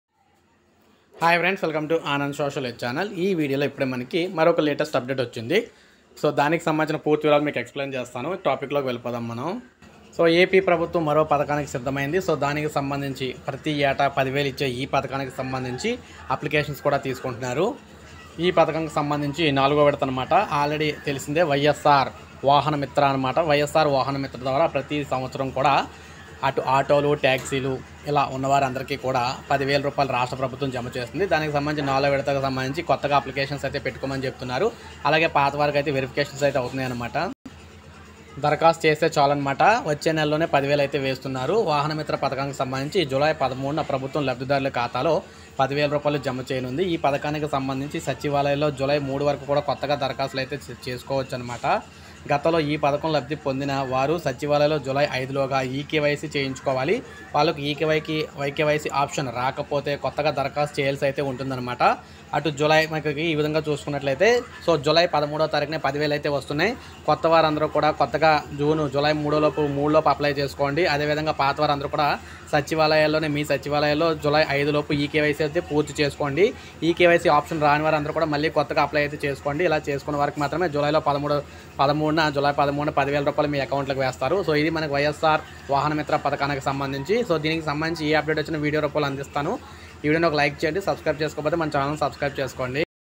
జూలై 13న వైయస్సార్ వాహన మిత్ర 10000/- జమ ysr vahana Mitra 4th installment.
hello everyone today update is AP government announced that YSR vahana Mitra scheme amount released